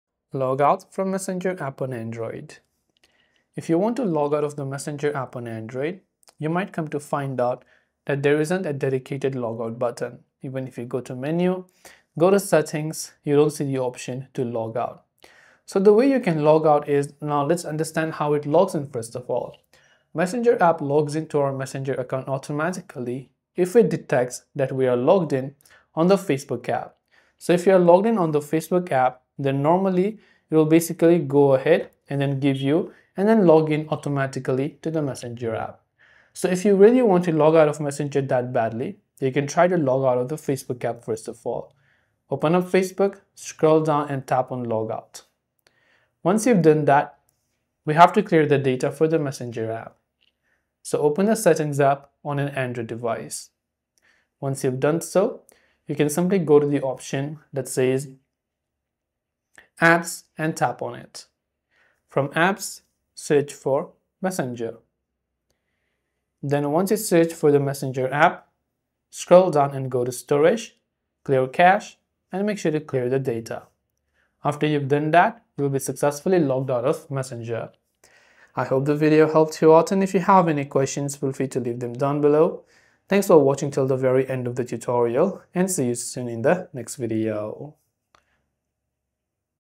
Logout From Messenger App on Android ! 2025
➜ If you're trying to log out of the Messenger app on Android in 2025, you’ve probably noticed that there's no straightforward "Log Out" button inside the app. That’s because Messenger on Android is deeply connected to your Facebook account and uses your Facebook login credentials automatically—especially if you're logged into the Facebook app on the same device.

To effectively log out of Messenger, the first step is to log out of the Facebook app itself. Open the Facebook app, tap the menu icon, scroll down, and select "Log Out." This will disconnect your Facebook session, which Messenger depends on to stay logged in.

After you've logged out of Facebook, the second step is to clear Messenger's app data. This action removes any saved login information and forces the app to show the login screen the next time you open it. To do this, go to your Android phone's Settings, tap on "Apps," and search for "Messenger." Once you're in Messenger's app settings, tap on "Storage." Then tap both "Clear Cache" and "Clear Data." This will effectively log you out of Messenger.

Keep in mind that clearing app data won’t delete your messages or conversations, as all chat history is stored in the cloud and linked to your Facebook account. When you log back in, your messages will reappear.

This is the most reliable way to log out of Messenger on Android, as there's currently no built-in log-out feature in the app itself. If you're using Messenger Lite or a third-party version, the steps might differ slightly, but clearing data will almost always do the trick.

Timestamps:
0:00 Introduction
0:12 Why there's no log-out button
0:30 Step 1: Log out of the Facebook app
0:49 Step 2: Clear Messenger app data
1:17 Conclusion